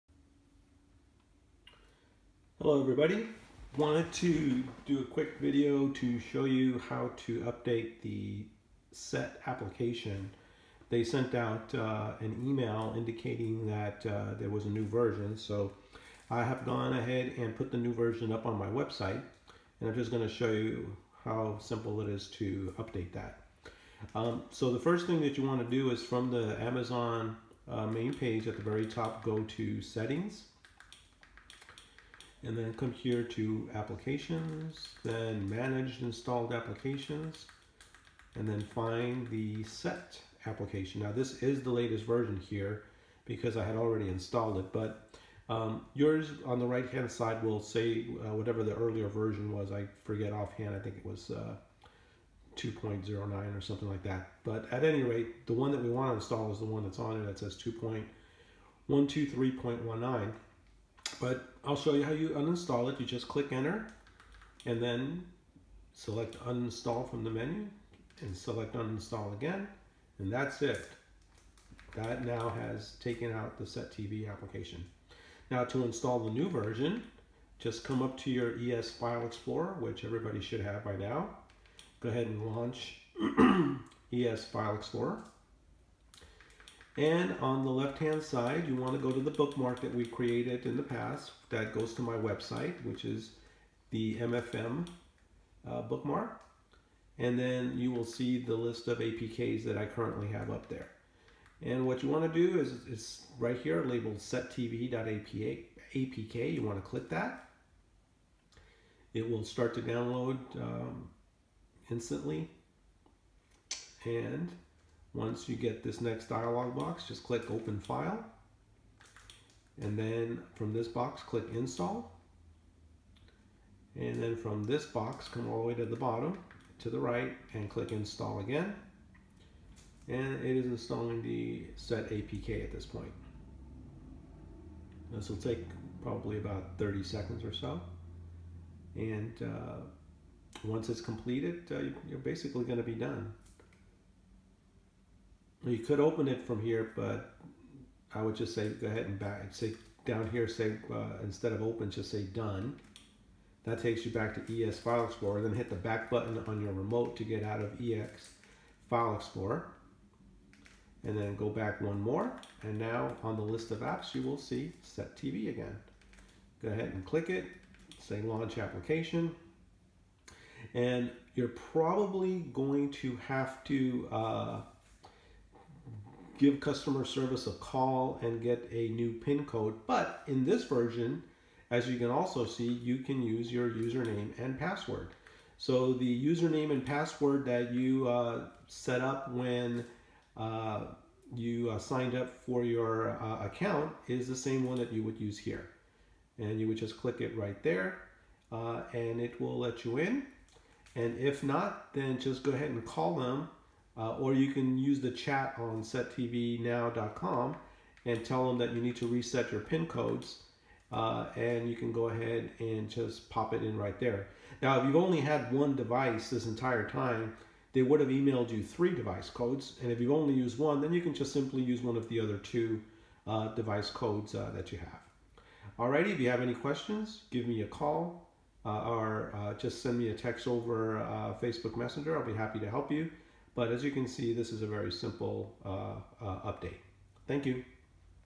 SetTv App Update
This shows how to update to the latest version of the Set TV apk